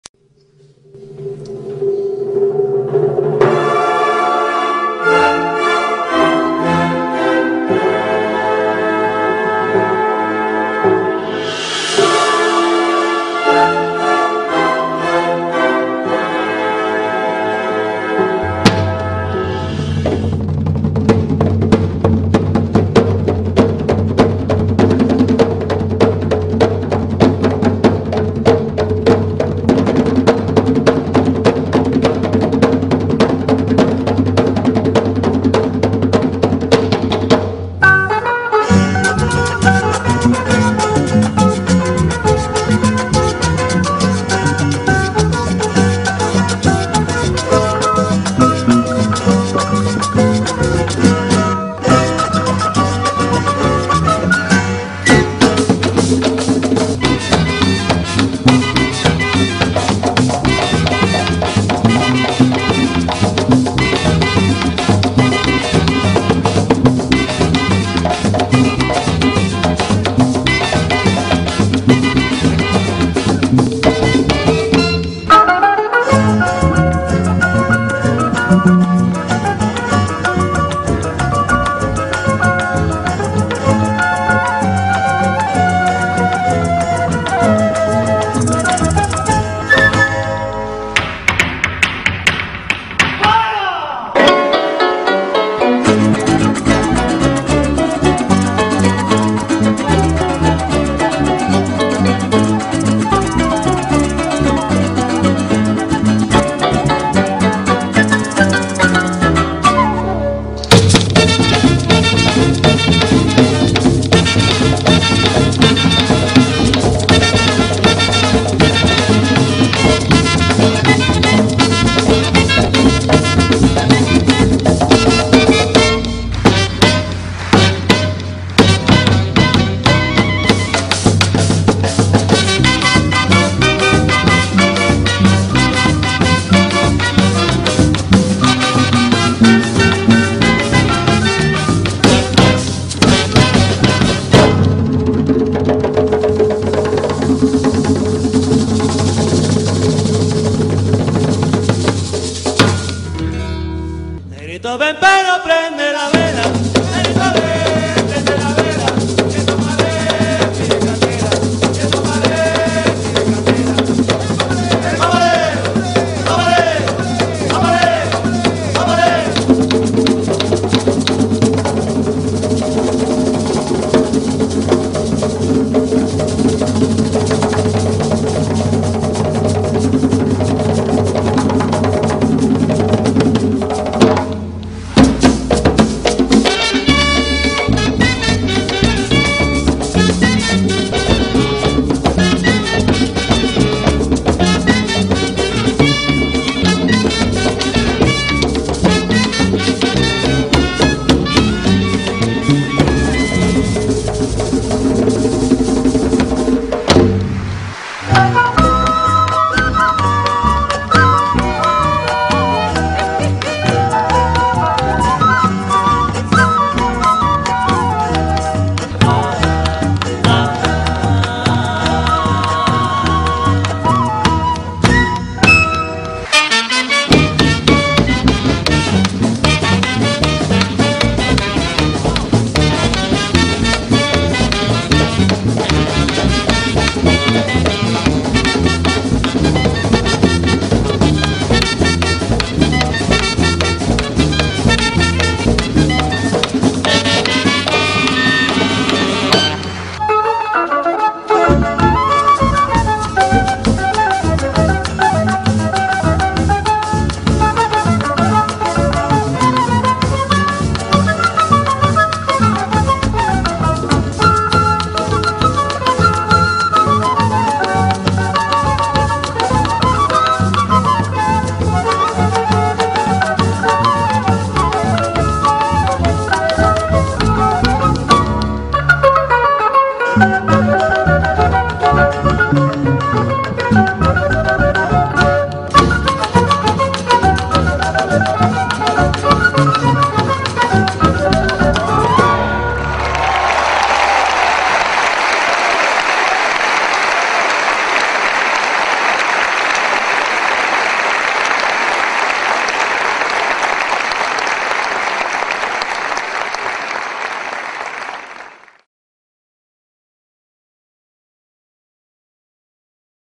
Sonia Osorio - Embajadora de Colombia
Regalo muy especial de su hijo Rodrigo Obregón para el Director de la Fundación La Nueva Esperanza de Secuestrados Abogado Gustavo Adolfo Muñoz Roa.